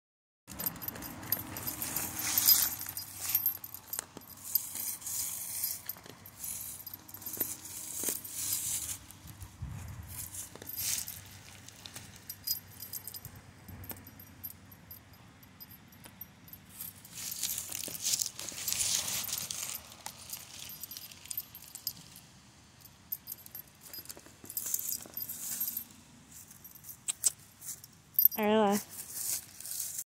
6 month old Mastweiler
Earla doing one of her favorite things, digging.
 She gave us a real scare last week. She got really sick. We're really thankful she pulled through.